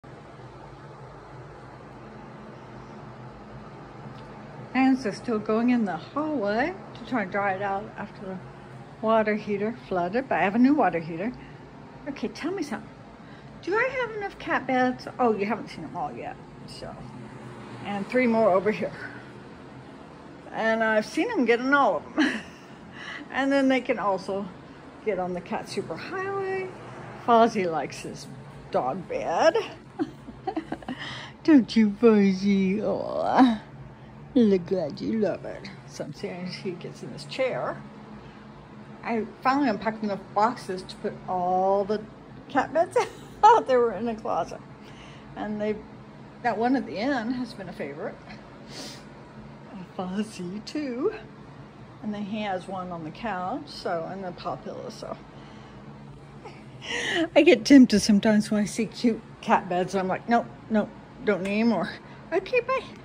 Do I have enough cat beds?
I finally unpacked enough moving boxes to be able to lay out all my cat beds that were stashed in a closet.